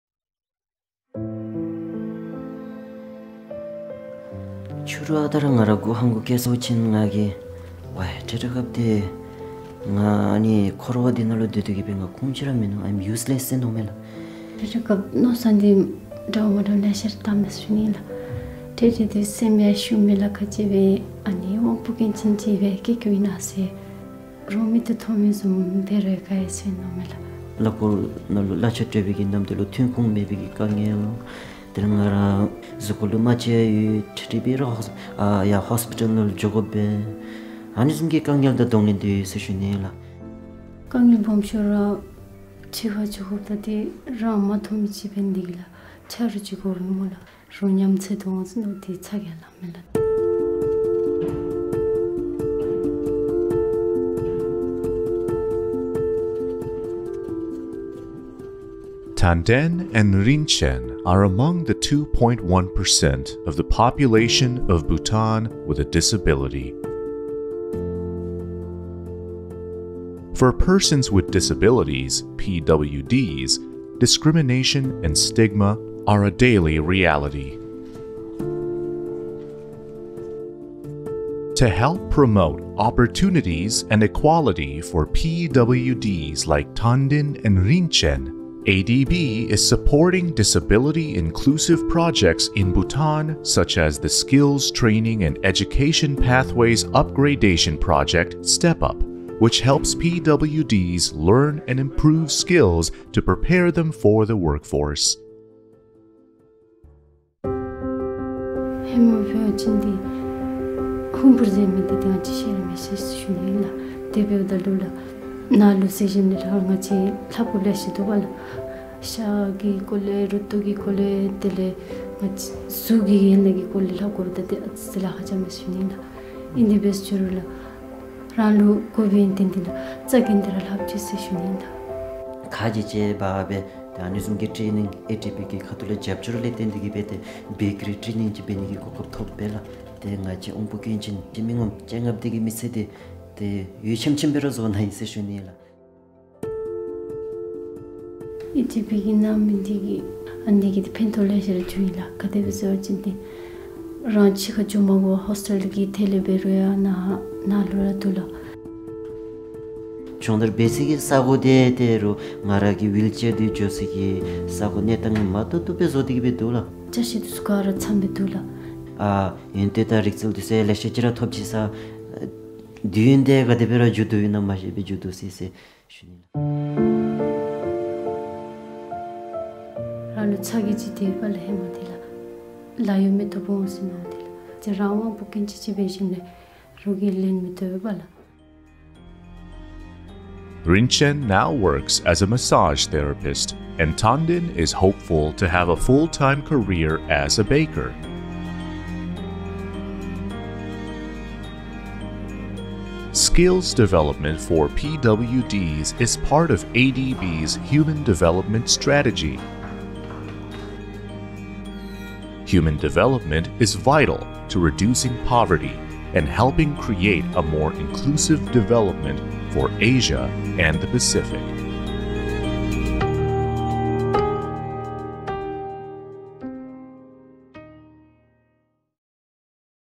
Building Skills of Persons with Disabilities, Transforming Lives
The government of Bhutan recognizes the importance of skills development for job quality and is committed to improving the technical and vocational education and training (TVET) sector. ADB’s Skills Training and Education Pathways Upgradation Project (STEPUP) is helping Bhutan expand and upgrade TVET system for modern, diversified, and job-oriented skills development. An important part of this project is to make the TVET program inclusive for persons with disabilities (PWDs).

Overall, ADB’s support aims to increase skilled human capital for equitable socioeconomic development in Bhutan.

Chapters
0:00 - Building Skills   of Persons with Disabilities, Transforming Lives
1:22 - ADB’s Skills Training and Education Pathways Upgradation Project (STEPUP) is supporting disability-inclusive   development in Bhutan
3:15 - Skills development for persons with disabilities is part of ADB’s human development strategy to reduce poverty and create a more inclusive development for Asia and the Pacific